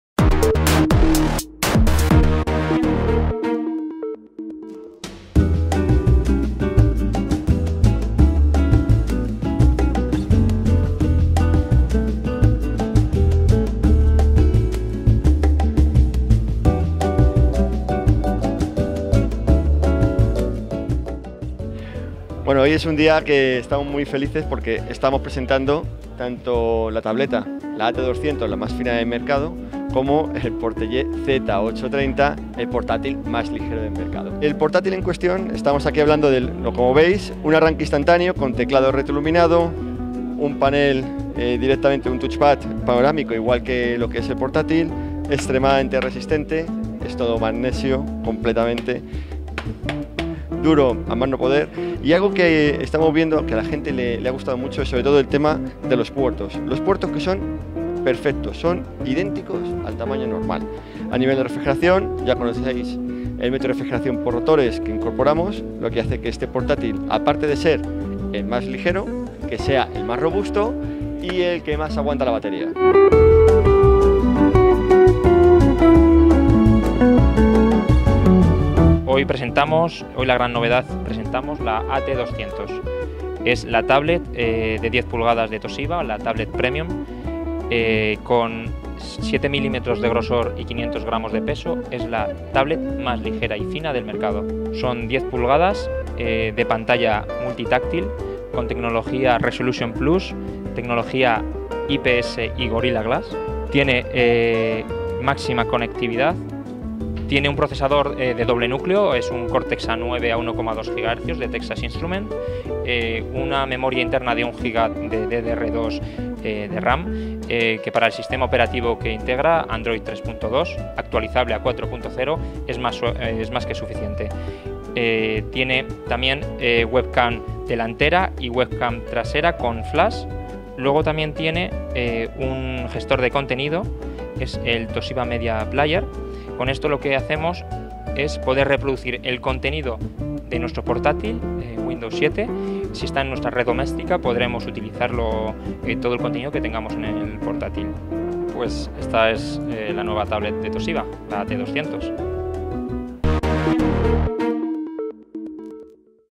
Presentación Toshiba AT200 parte 2
Presentación en el 40 Café de Gran Vía de la nueva tableta de Toshiba, la AT200.